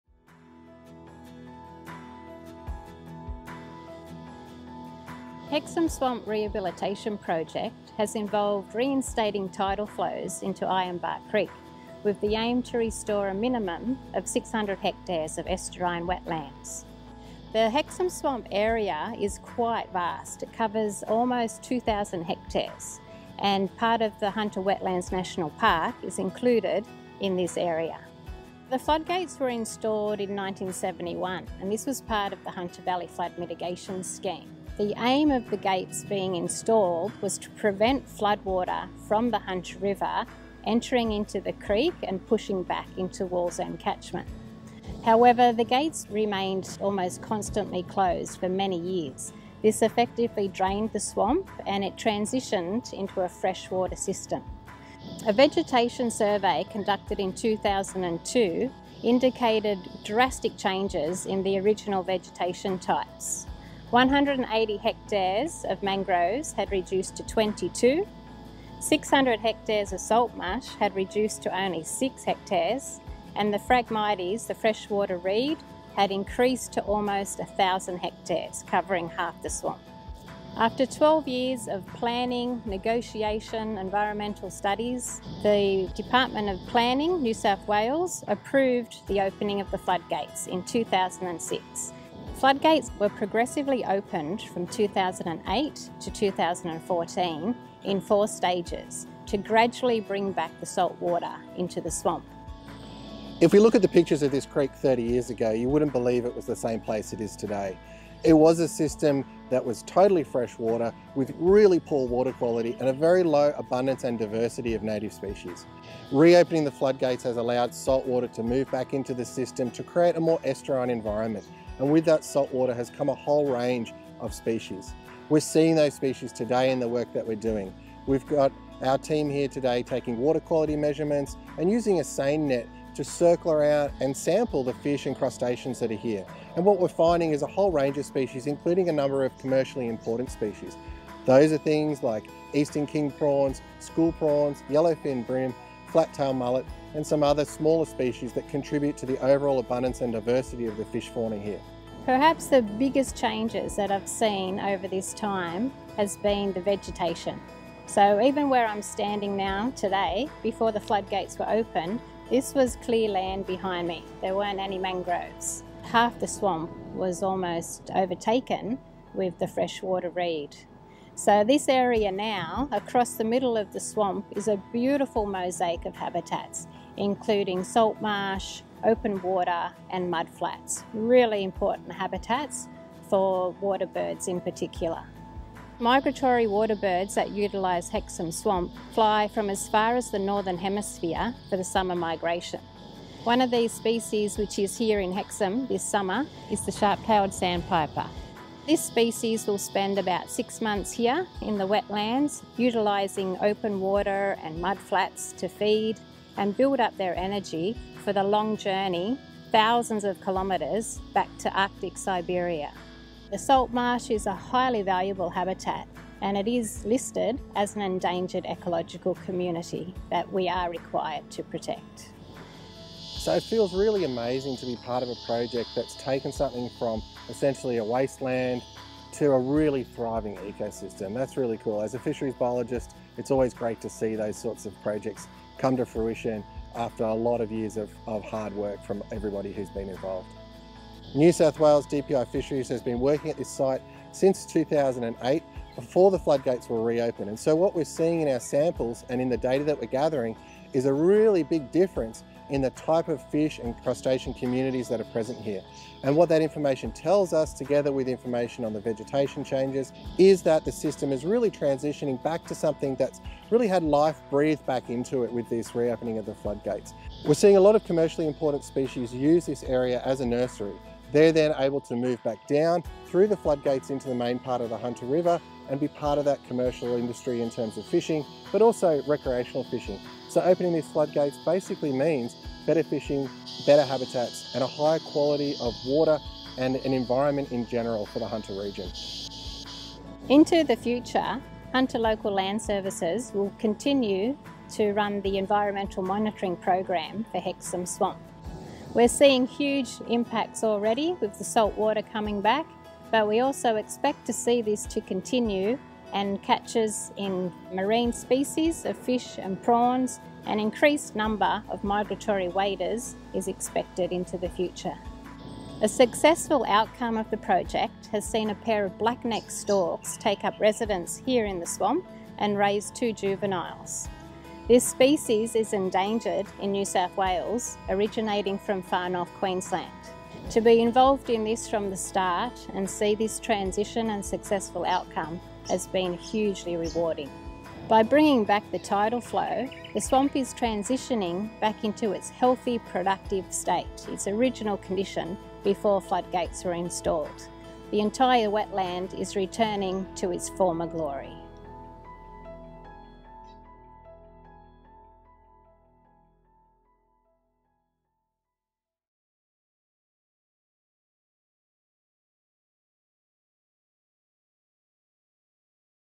HEXHAM SWAMP REHABILITATION PROJECT- restoring tidal inundation to improve estuarine wetland habitat
The Hexham Swamp Rehabilitation Project involved reinstating tidal flow into Ironbark Creek with the aim to restore a minimum 600 hectares of estuarine wetlands. The Project was initiated over twenty-five years ago and environmental monitoring continues today, managed by Hunter Local Land Services in accordance with NSW government requirements. The swamp covers a vast area of almost 2000 hectares and includes part of the Hunter Wetlands National Park. The project is successfully bringing back a range of wetland types including mangroves and saltmarsh - an endangered ecological community that had been overtaken by freshwater vegetation. This transition is restoring nursery habitat for juvenile fish and prawns and provides important habitat for waterbirds, including migratory shorebirds that fly from the northern hemisphere every year for the summer migration.

The project has been a successful multi-partnership approach and Hunter Local Land Services would like to acknowledge NSW DPI Fisheries for appearing in the video. Hunter Local Land Services acknowledges Lene Parashou for footage of the Sharp-tailed Sandpipers and Black-necked Storks.